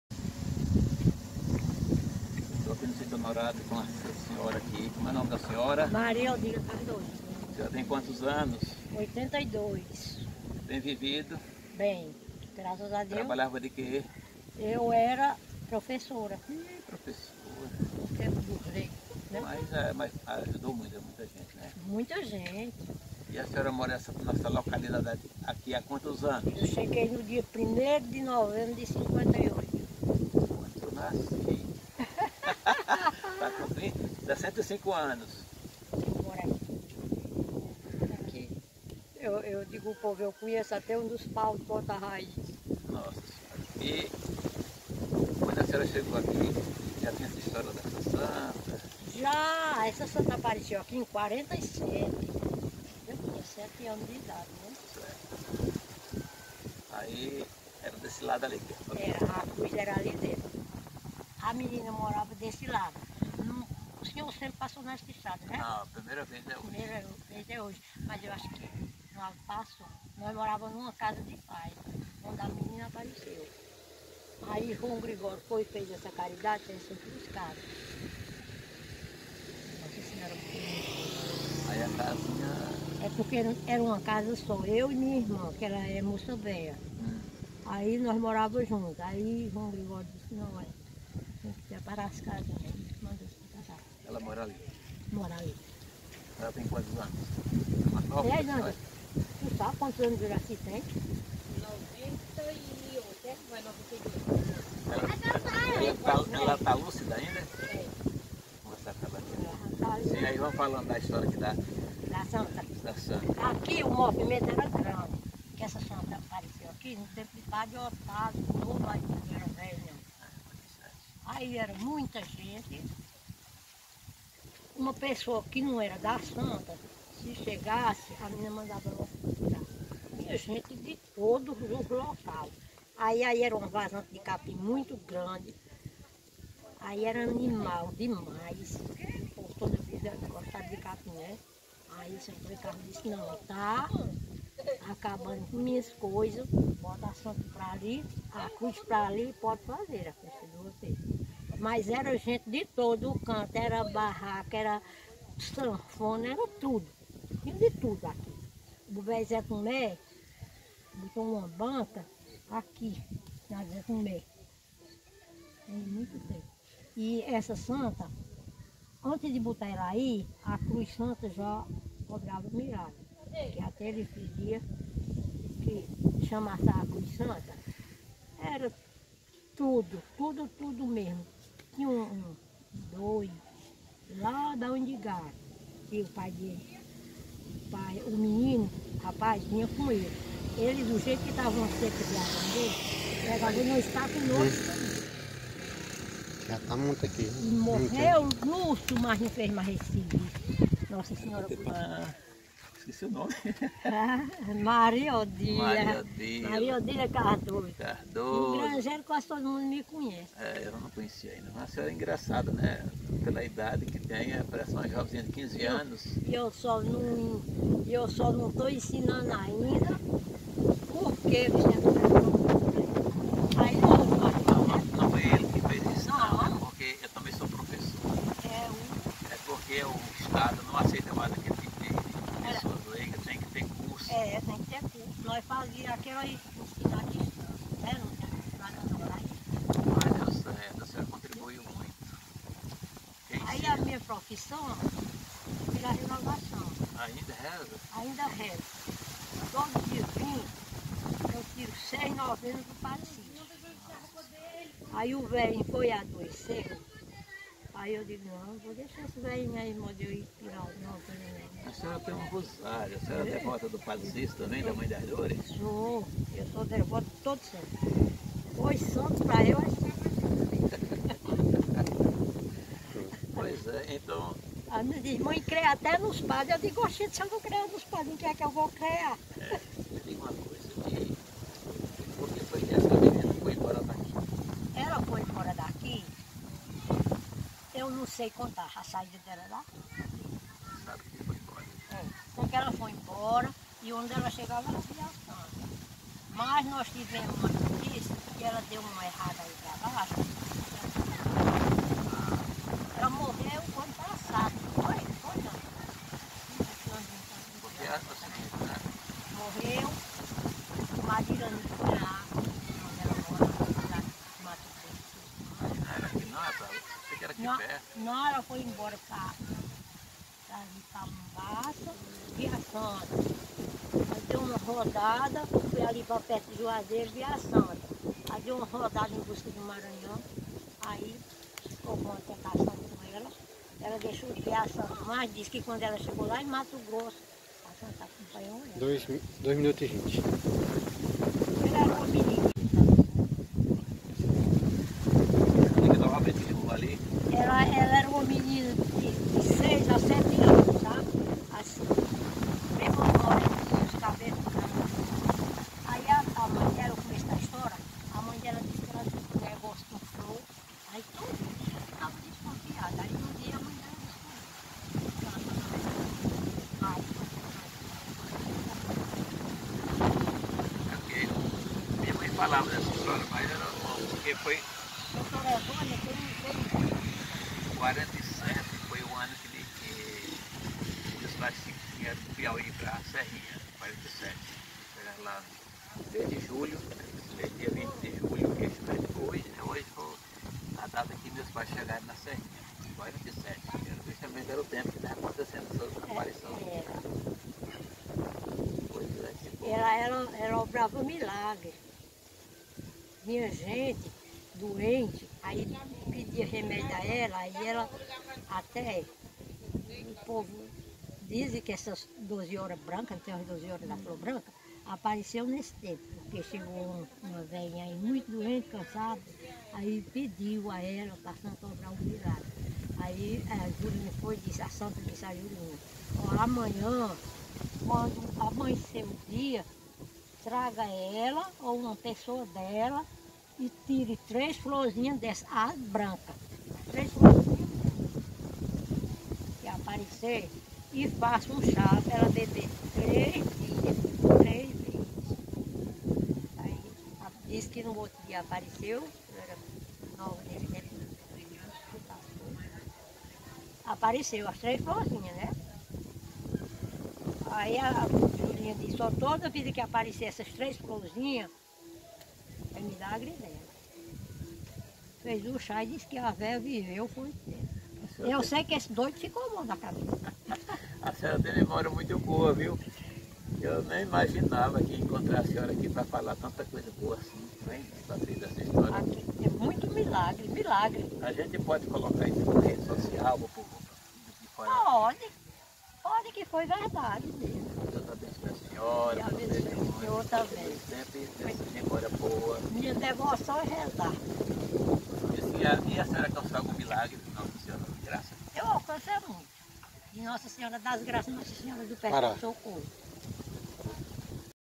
Meu amigo José Bacelar visita o Sítio Honorato e conversa com dona Maria Odila Cardoso
Dona Maria Odila, moradora do Sítio Honorato, fala sobre as manifestações de fé em torno da menina Júlia e as visões de Nossa Senhora das Graças